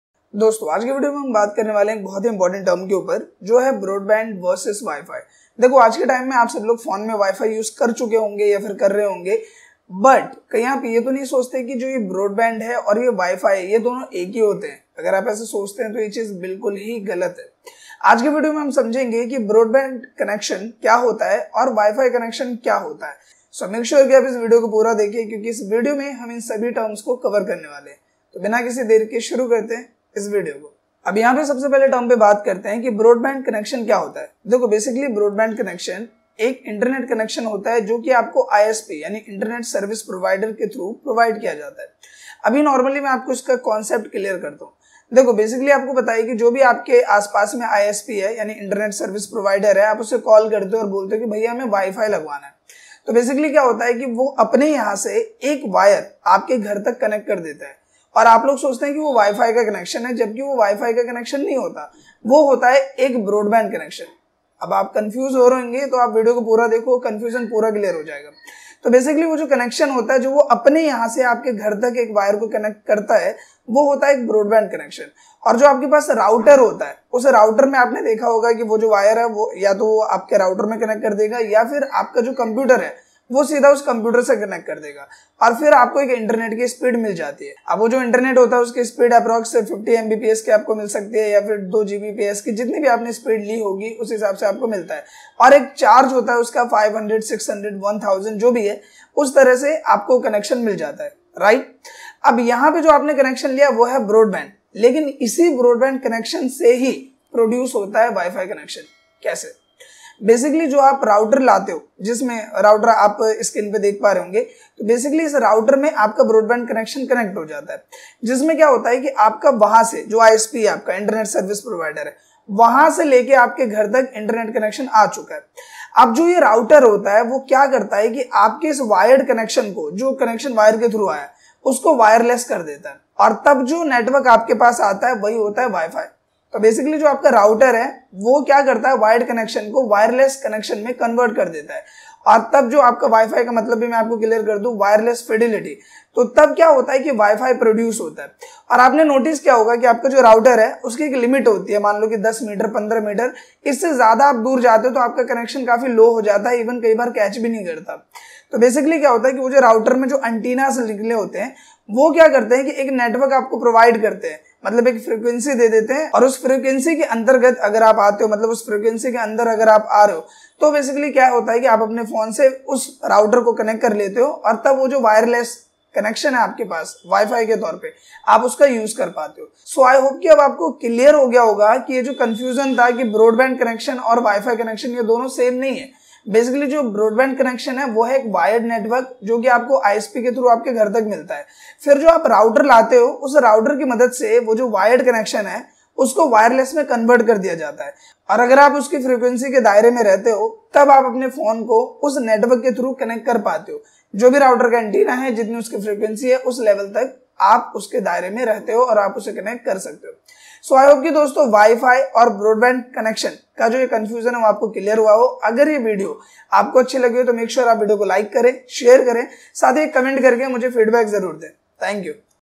wi-fi Vs Broadband || Difference between Broadband & Wi-fi || 99% लोग नहीं जानते
In this video, we will understand the difference between wi-fi and Broadband.
wi-fi is one of the common term we use on daily bases. but we don't know that from where the wi-fi produce.
Watch this video and understand it very easily.